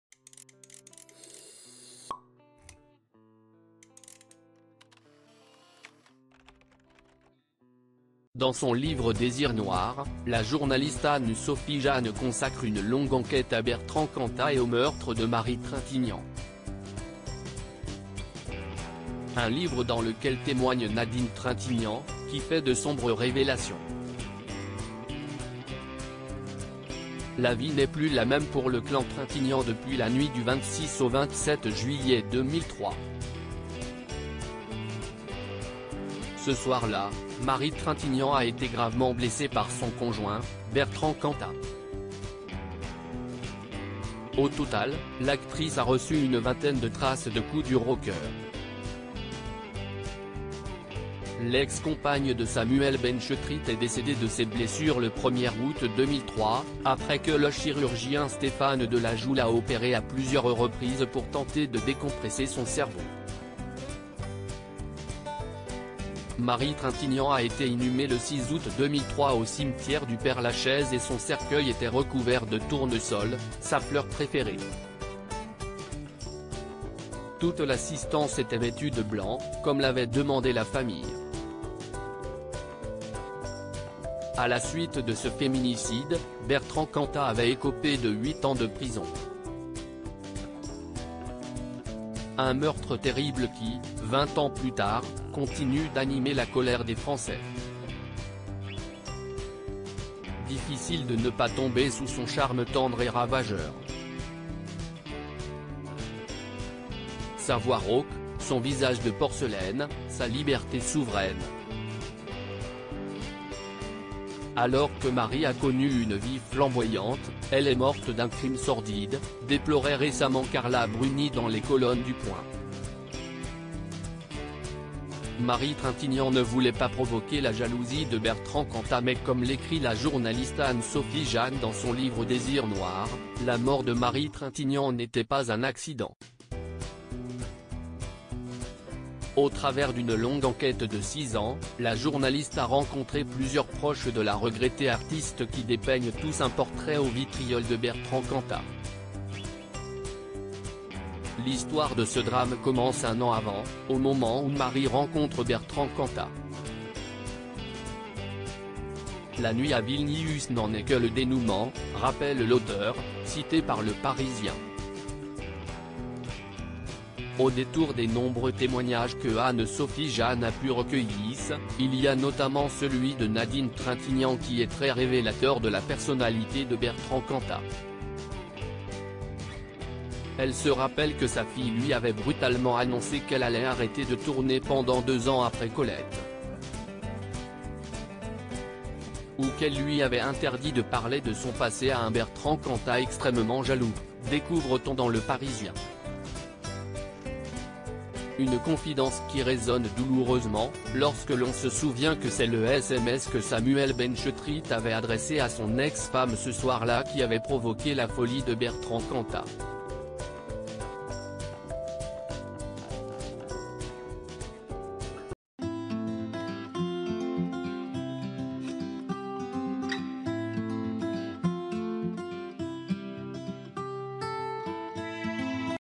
Bertrand Cantat “extrêmement jaloux” : ce que Marie Trintignant interdisait à sa mère Nadine Trintig
Bertrand Cantat “extrêmement jaloux” : ce que Marie Trintignant interdisait à sa mère Nadine Trintignant